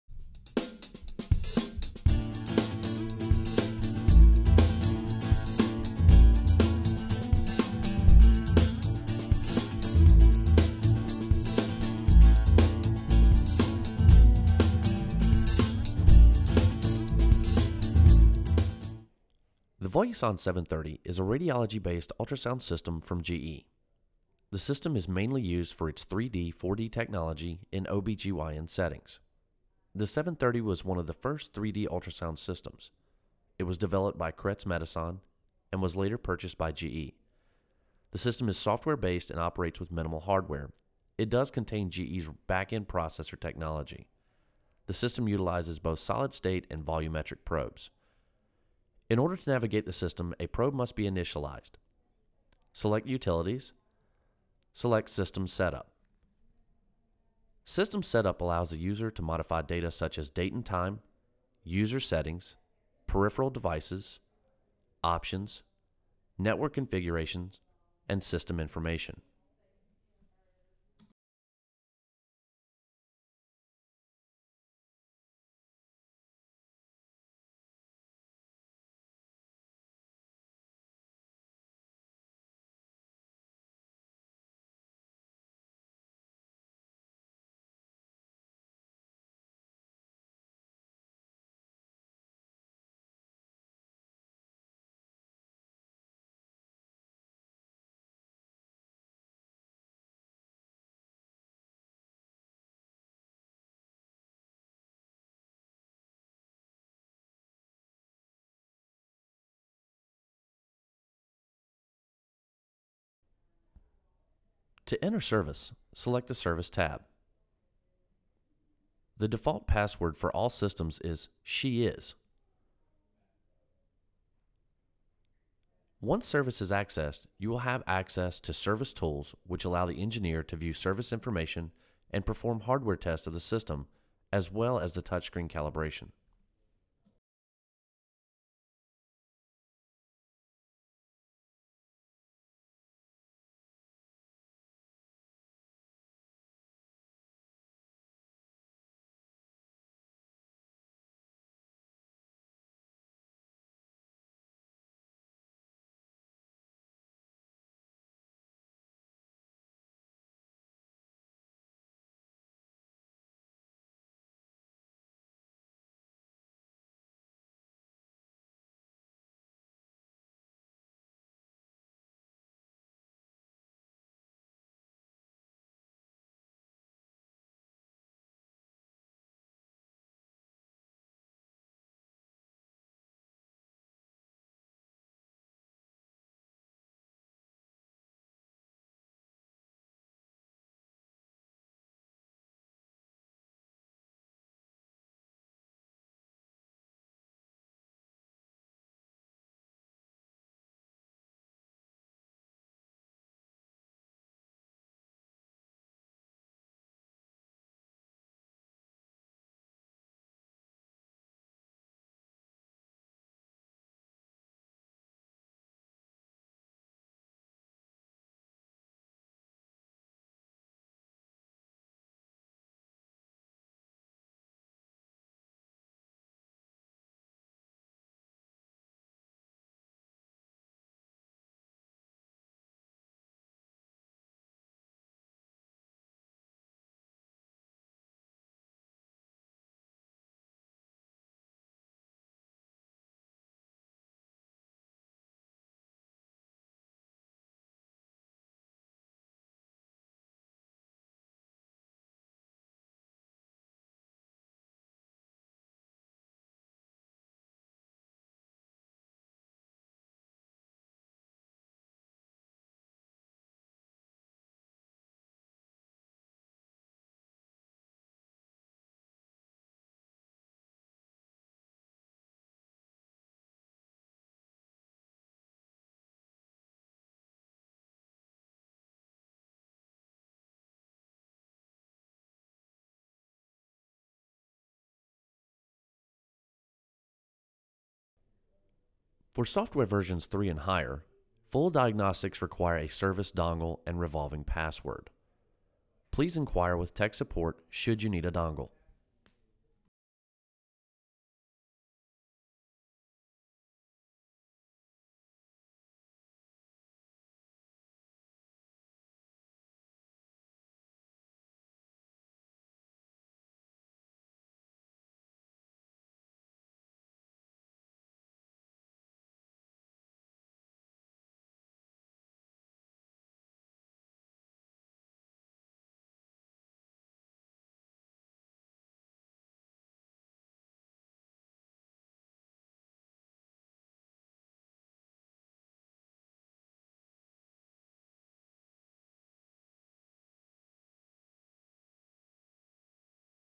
Voluson 730 Introduction
Introduction to the GE Voluson 730 Ultrasound system, to include essential backup procedures.